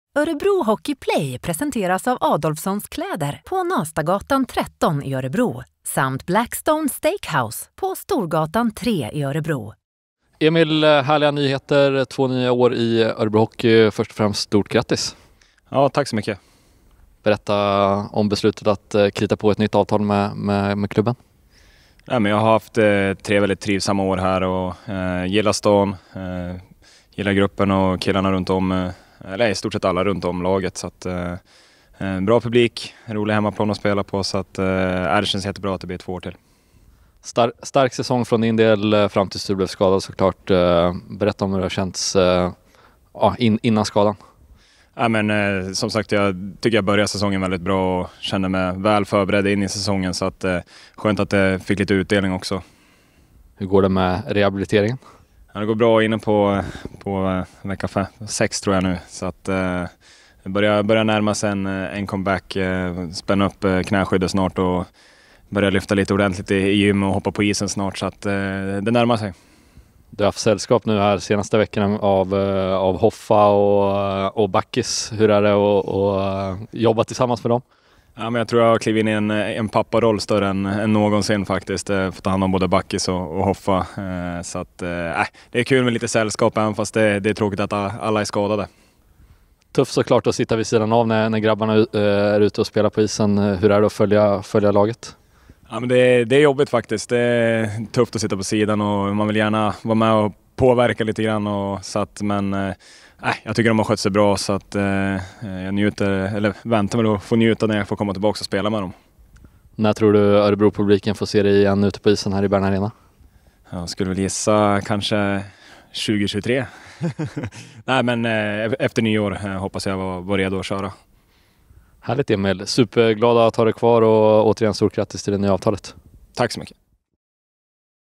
"En rolig hemmaplan att spela på" - Emil Larsson förlänger med Örebro Hockey
Emil Larsson berättar om sitt nya tvåårskontrakt med Örebro Hockey.